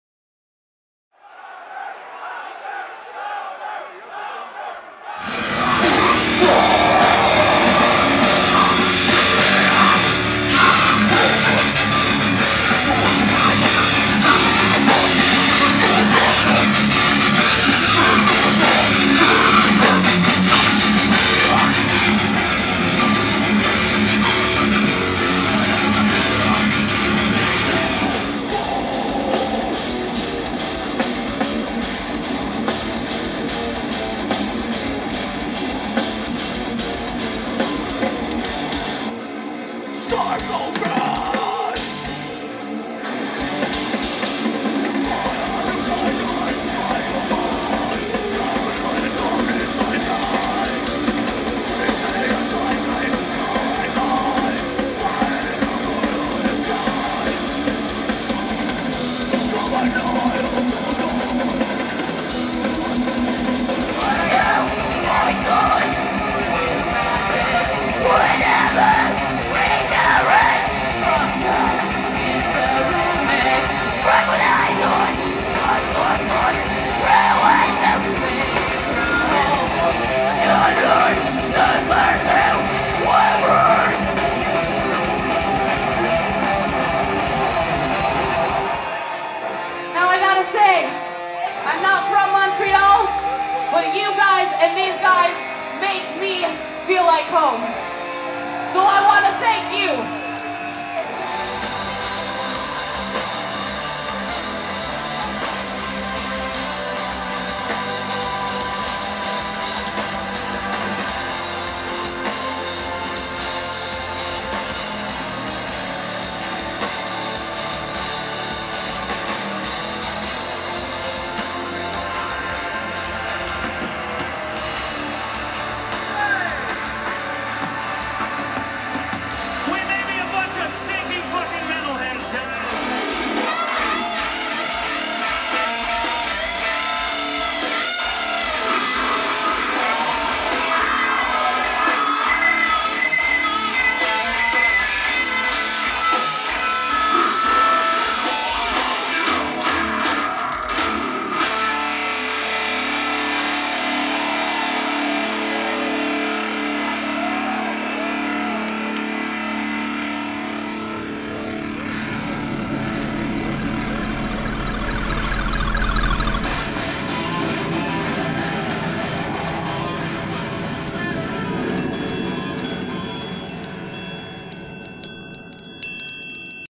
Heavy Montréal 2015
I got to experience the Heavy Montréal and not only did I get some great photos which you can spot at Worldmind.info
But I got some great footage as well and here is a highlight of my Festival Experience.